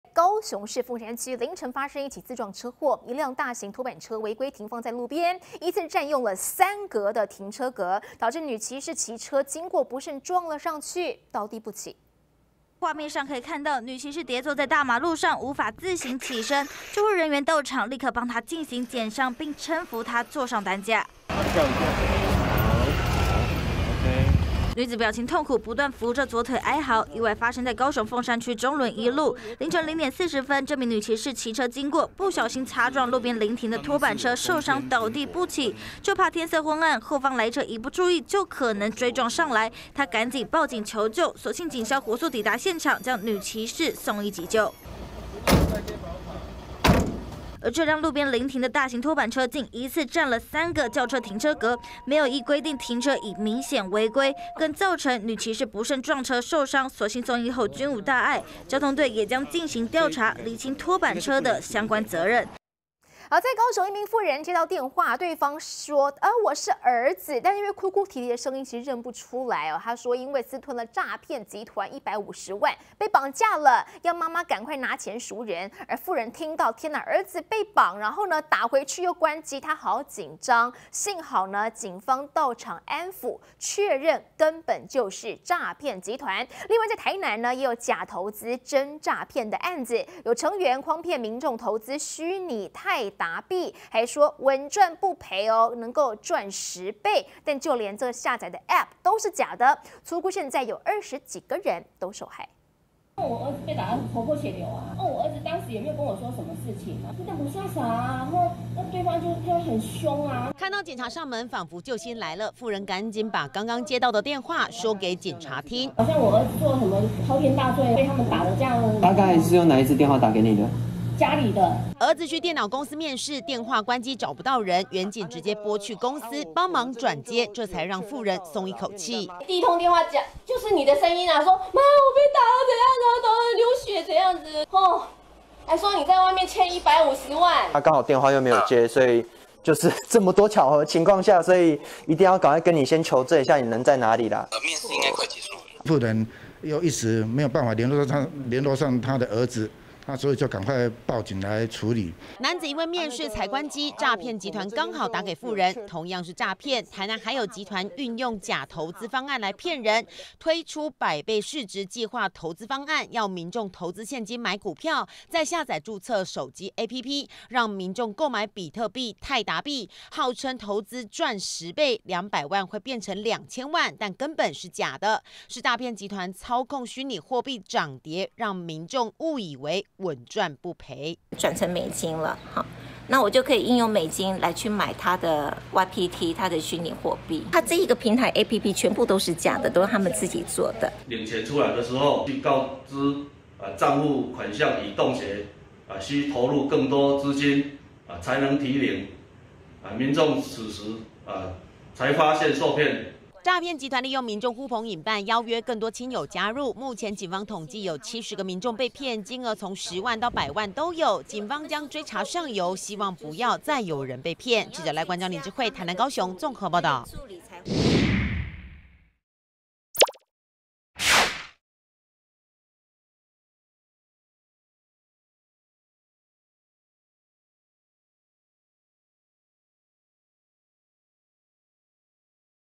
【每日必看】拖板車違停害騎士撞上倒地送醫 老哏!媽 我被綁架...婦險匯150萬 @中天電視CtiTv 20210408
高雄市鳳山區，凌晨發生一起自撞車禍，一輛大型拖板車違規停在路旁，一次佔用"三個"汽車停車格。導致女騎士騎車經過不慎撞上，倒地不起，嚇得她立刻報警求救。

高雄一名婦人接到電話，對方自稱是他的兒子，哭哭啼啼的說因為私吞詐騙集團150萬，人被綁架，要婦人拿錢贖人，婦人馬上打電話給兒子卻又關機，轉而報警，幸好警方到場安撫，確認就是詐騙，另外台南也有假投資的詐騙案，成員誆騙民眾投資虛擬泰達幣，還說穩賺不賠，可以賺十倍，但其實連下載的APP都是假的，初估有70個人受害，遭騙金額從十萬到上百萬。